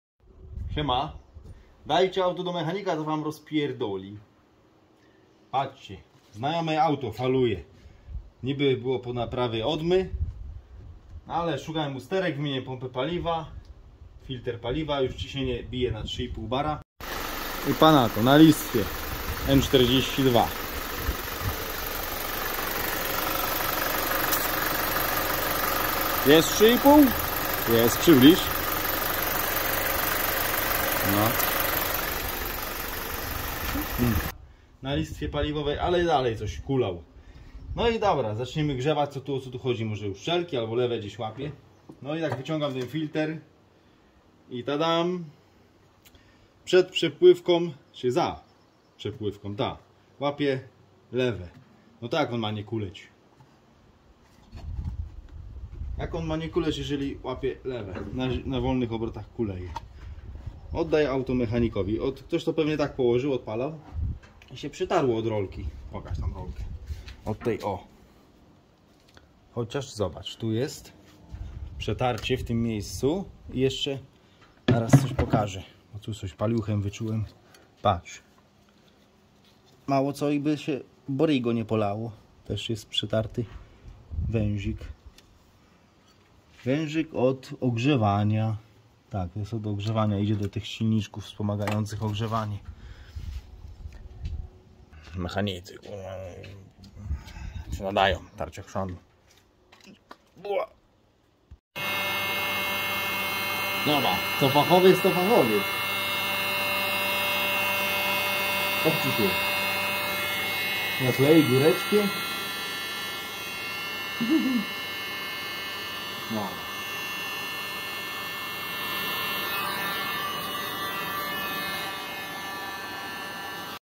BMW E46 silnik N42 problem z falującym silnikiem, czasami potrafił zgasnąć. Problem rozwiazany.
Po odebraniu od mechanika który robił pierścienie i komplet głowice okazało się że auto dalej faluje i gaśnie. Zostaly zrobione adaptacje silnika a później szukanie usterek których komp nie widział , typu pompa paliwa i filtr paliwa/ nieszczelności dolotu za przepływomierzem. Problem rozwiązany.